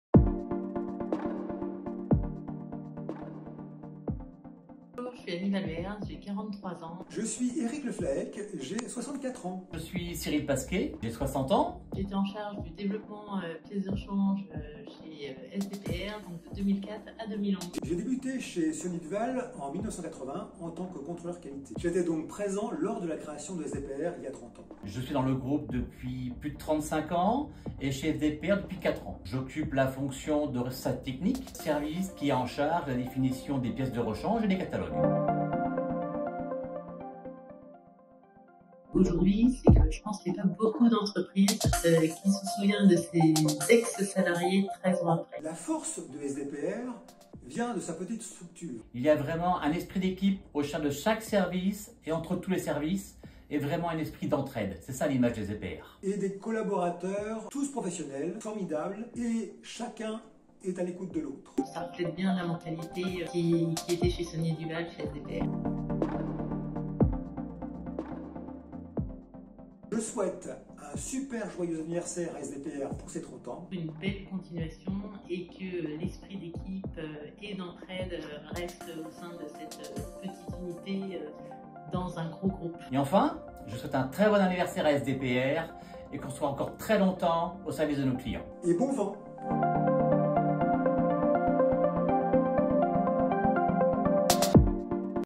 30 ans de Service Technique
A l'occasion des 30 ans de Saunier Duval Pièces de Rechange, nous mettons à l'honneur les services qui font la force de SDPR !

Focus sur notre service Technique pièces de rechange : découvrez les témoignages des collaborateurs, anciens et actuels qui ont contribué à l'histoire de notre société.
Depuis 30 ans, de la création des références, à la mise à disposition des vues éclatées, le service technique est toujoursàvoscôtés.

Un grand merci à Annie Valverde, Eric Le Flahec et Cyrille Pasque pour leur participation.